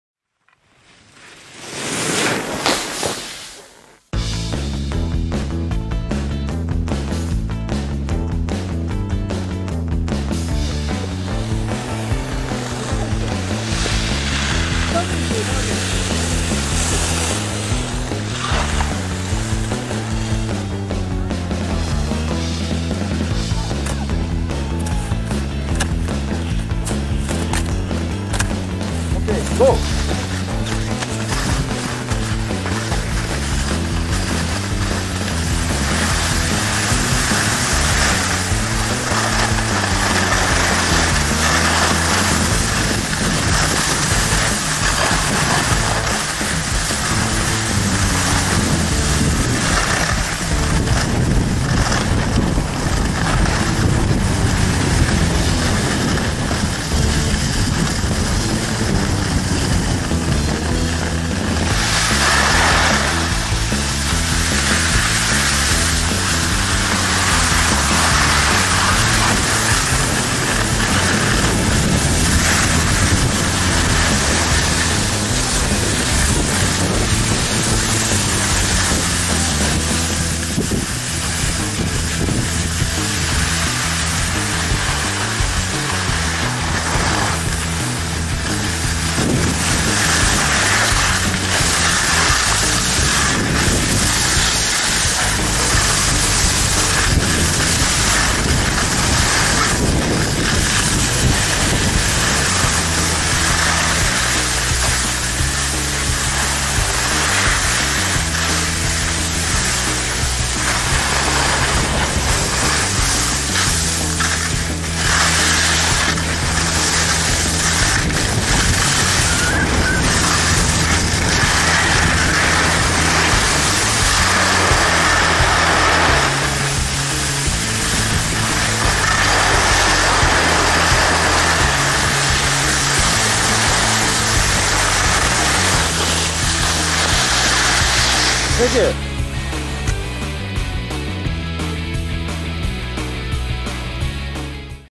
木曽福島スキー場/Kisofukushima skiing field 20200307
Enjoyed skiing at Kisofukushima skiing field.
Condition was good.